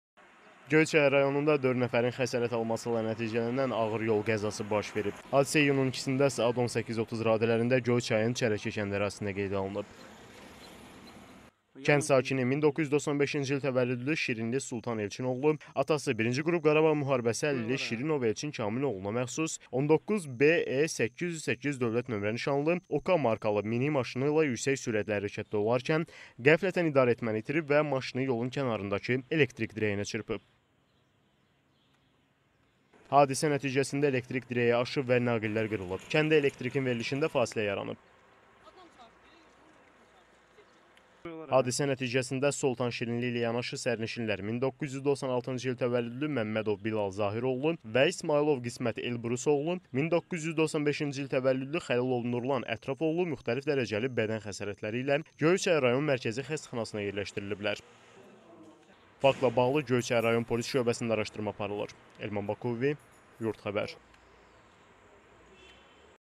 Qarabağ müharibəsi əlili qəza törətdi
Qarabağ müharibəsi əlili qəza törətdi: Onun idarə etdiyi maşın elektrik dirəyinə çırpıldı, 4 nəfər xəsarət aldı